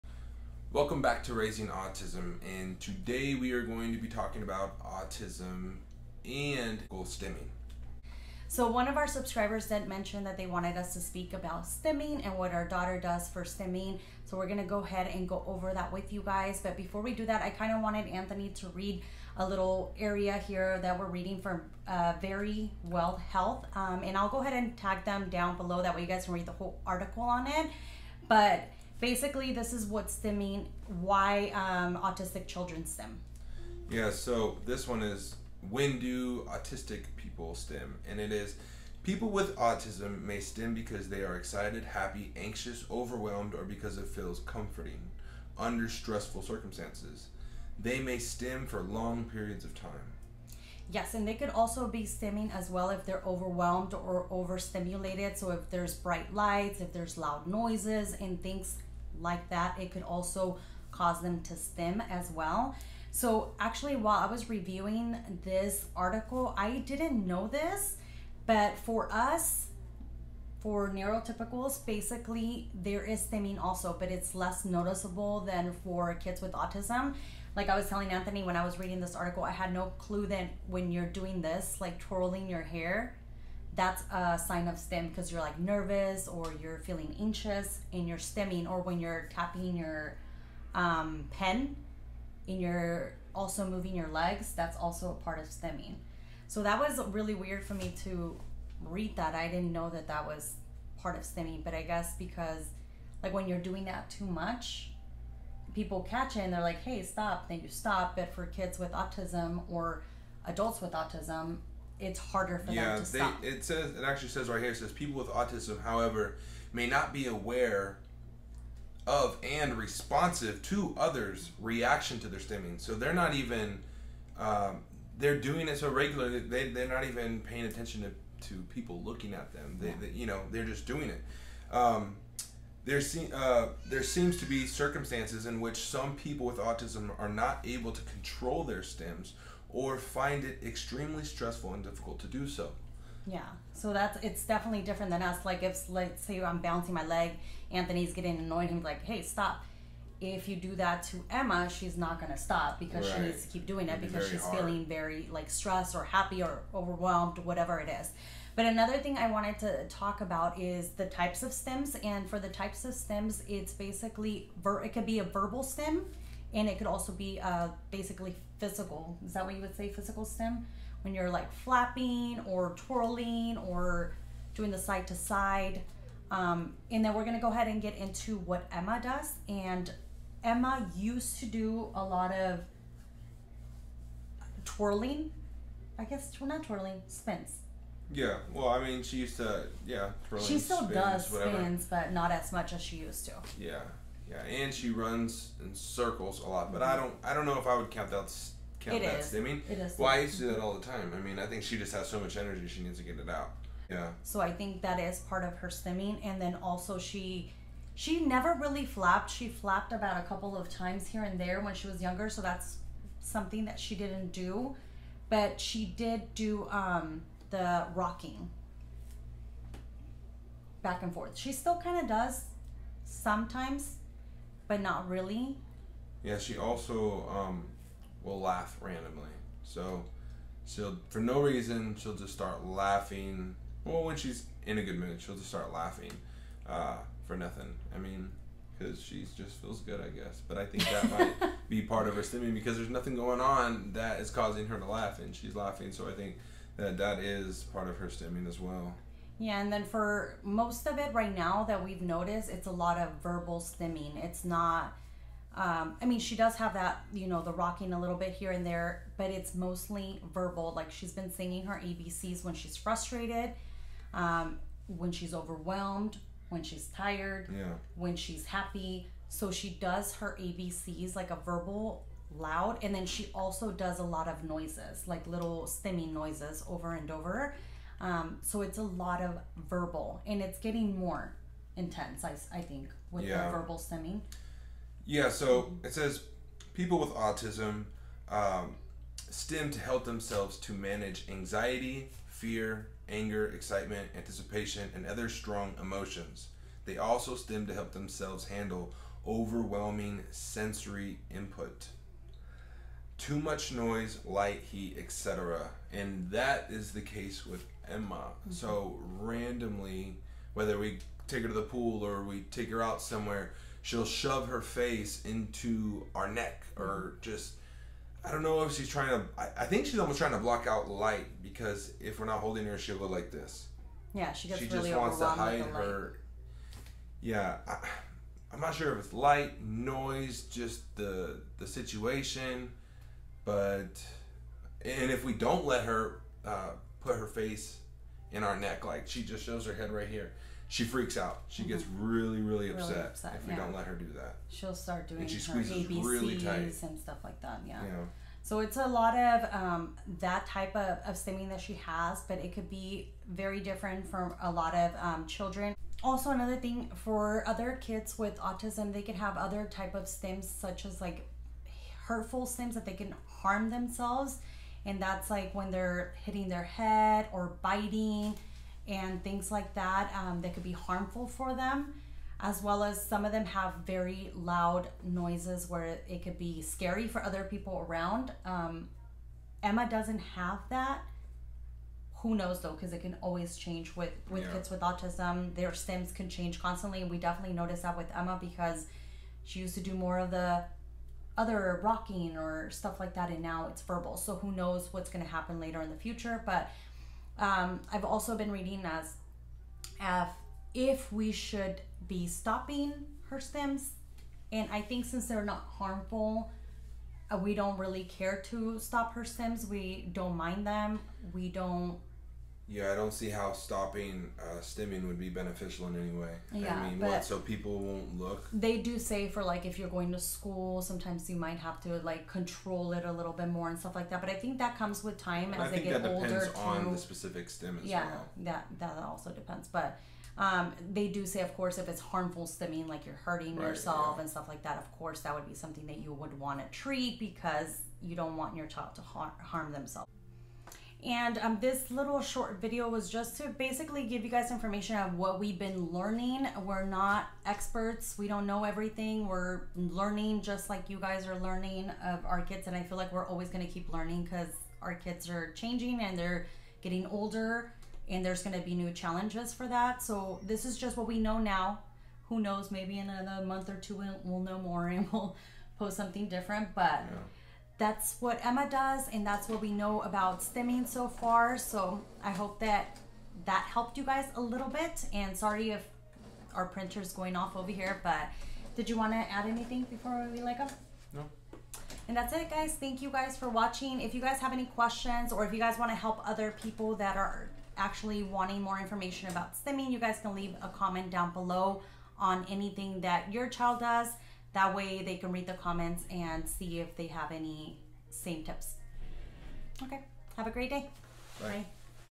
Signs Of Stimming in Toddlers | STIMMING BEHAVIORS
HI loves todays video we talk about stimming self stimmulation in toddlers
stimming behaviors with children or toddlers in the autism spectrum disorder

Signs of autism with home footage of our daughter Autism Early Signs | Toddler Signs Of Autism With Footage

This channel is the way I can help my husband support the family

Veronica Gutierrez

Iappreciate the support